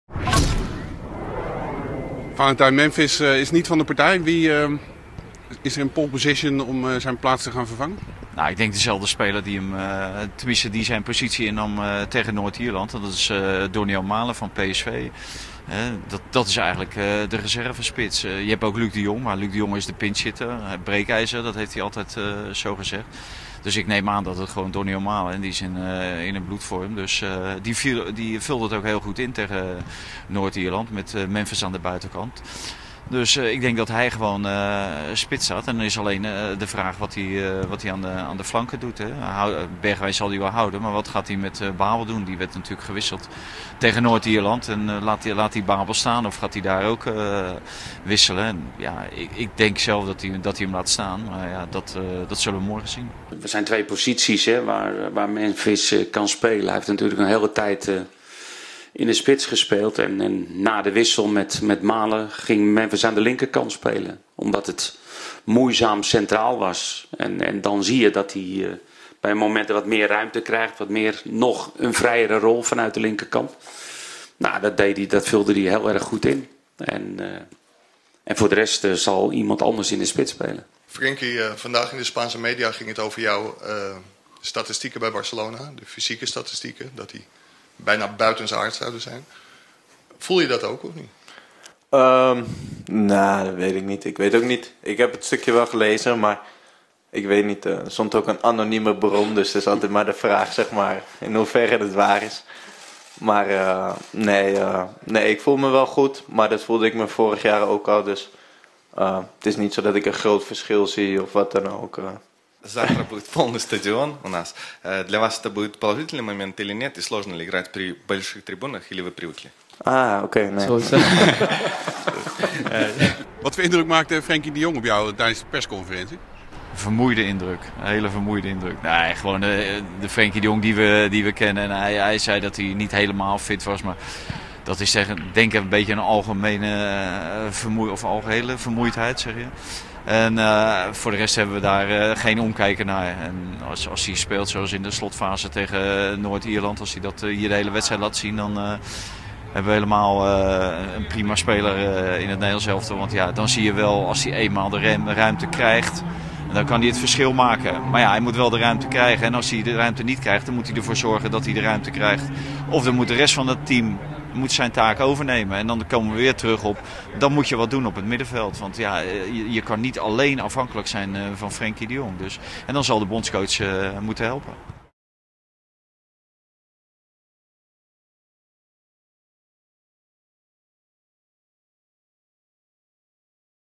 ‘Koeman moet Frenkie helpen’
Oranje moet het zondag doen zonder aanvaller Memphis Depay. Hij wordt vervangen Donyell Malen.